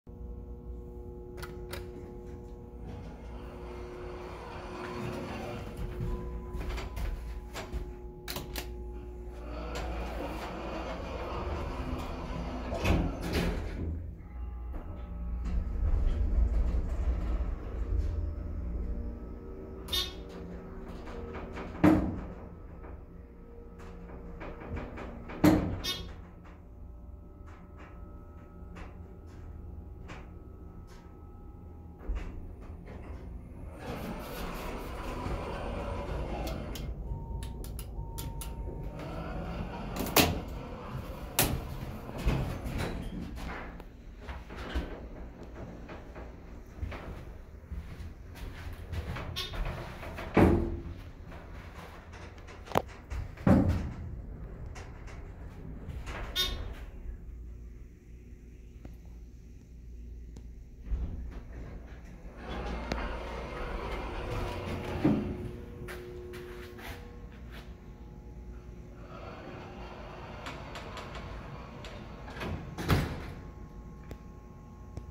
Dover Hydraulic Elevator @ Northpointe Medical Plaza, Berkley, MI
Filmed with Samsung Galaxy S9 on July 14, 2021

This is the elevator at the Northpointe Medical Plaza, a medical office building on Woodward. This is a nice brass Dover lift from the late 1980s. Its buttons have red lights.

Floors served: 3
Manufacturer: Dover
Type: Hydraulic
Model: Oildraulic
Fixtures: Impulse
Capacity: 2500 lb
Year installed: 1989
Michigan serial number: 26784

Location: Northpointe Medical Plaza, 27901 Woodward Avenue (M-1), Berkley, Oakland County, Michigan, United States.